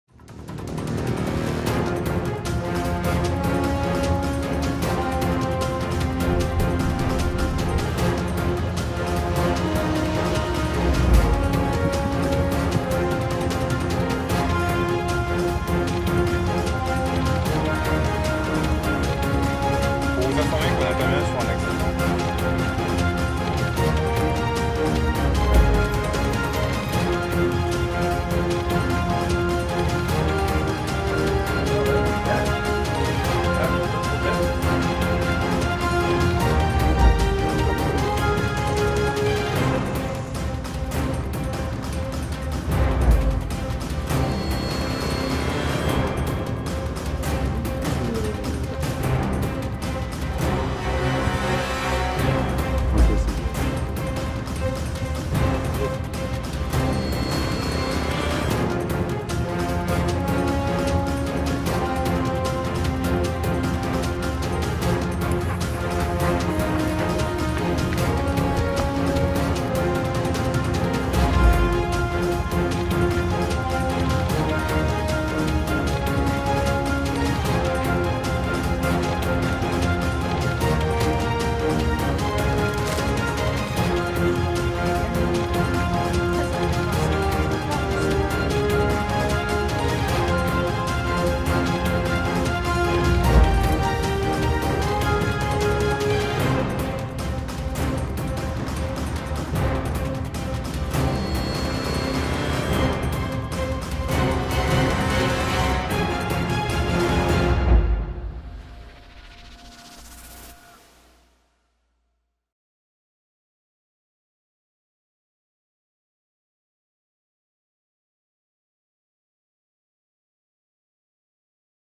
[CLIP] Les réservistes opérationnels et citoyens
Les réservistes de la gendarmerie, vous les connaissez ?
Près de 30 000 hommes et femmes qui portent l'uniforme et les valeurs de notre institution, et y exercent des missions très variées.
Alors, pourquoi pas vous ?